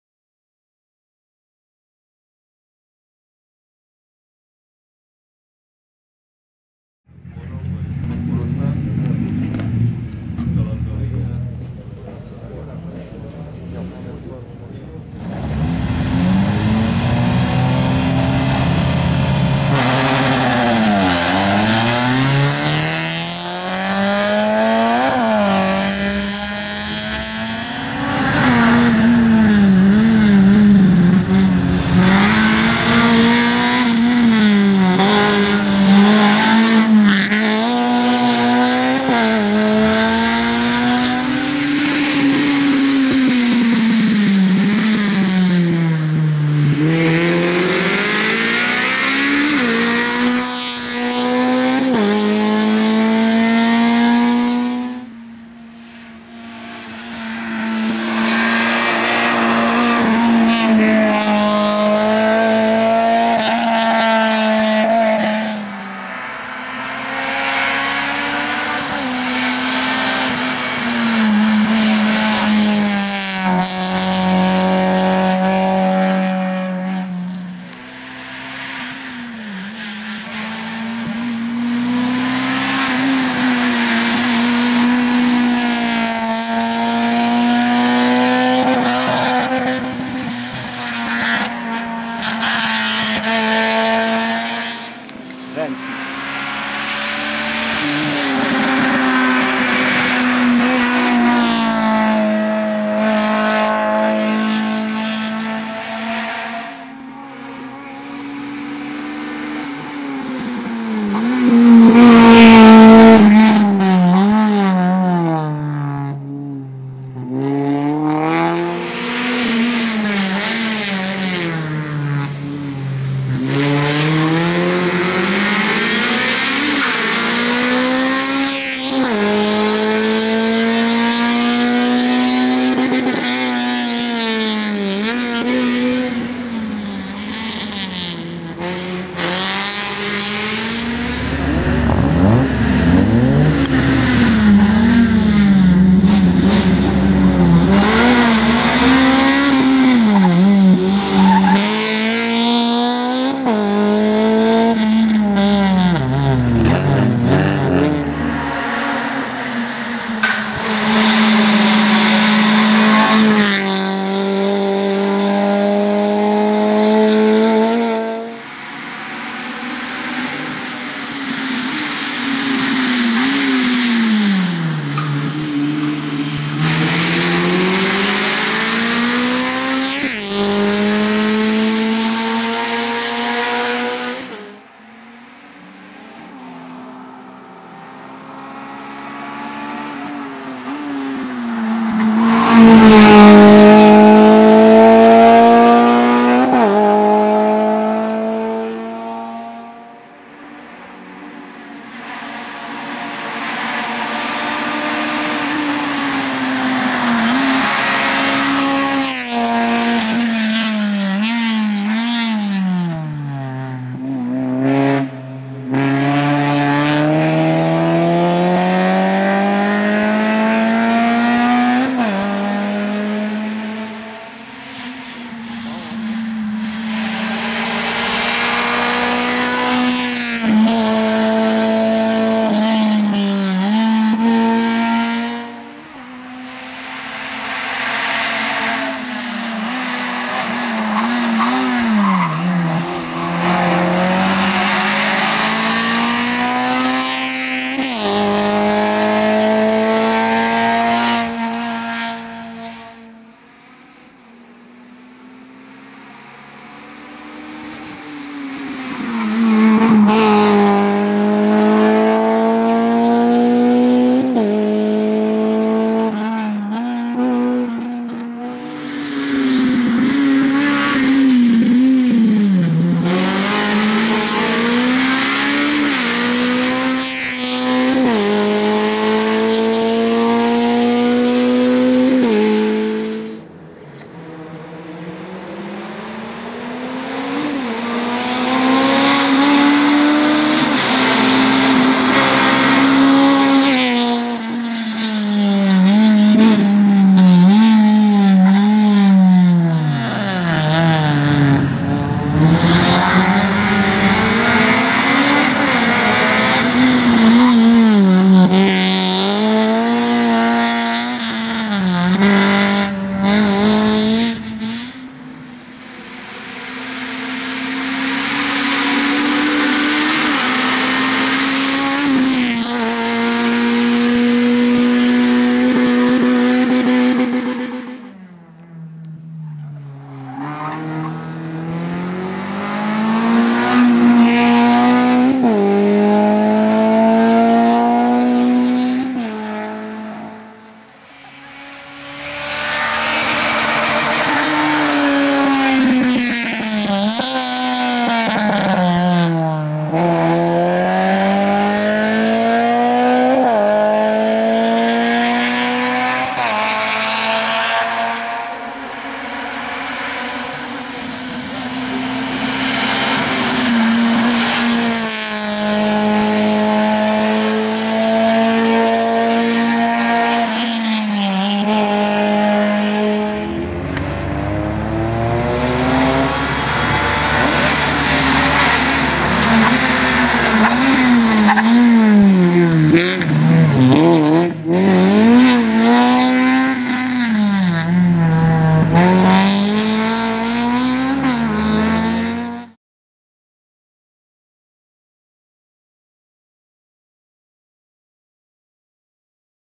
Huszár-Pozsicki Honda Civic Hídépítő Rallye Sprint 2018.
Huszár Péter és Pozsicki Zoltán kisfilmje,a 2018-as hídépítő rallye sprintről. /Lepold Sportvideo-2018./